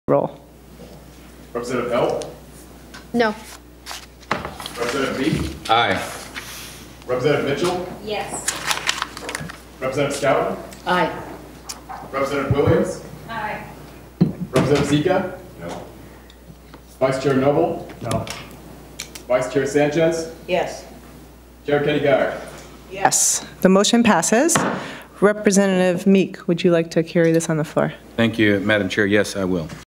B-roll: Oregon House committee passes rent-control bill
The Oregon House Committee on Human Services and Housing passes SB 608 rent control, no-cause evictions and tenant rights on a party-line vote. The Oregon House is scheduled to vote on the bill on Feb. 26, 2019.